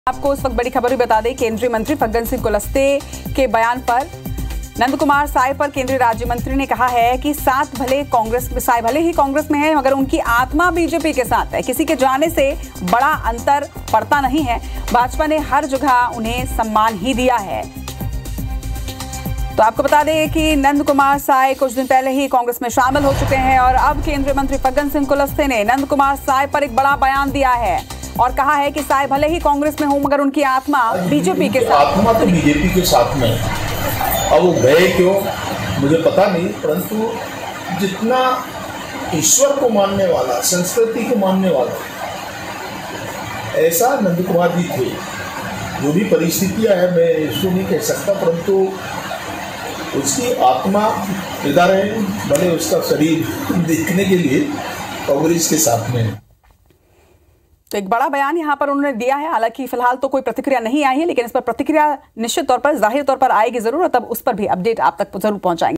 Faggan Singh Kulaste का बड़ा बयान | कहा- नंदकुमार साय कांग्रेस में हैं लेकिन उनकी आत्मा BJP के साथ
IBC24, केंद्रीय मंत्री Faggan Singh Kulaste का बड़ा बयान, कहा- नंदकुमार साय कांग्रेस में हैं लेकिन उनकी आत्मा BJP के साथ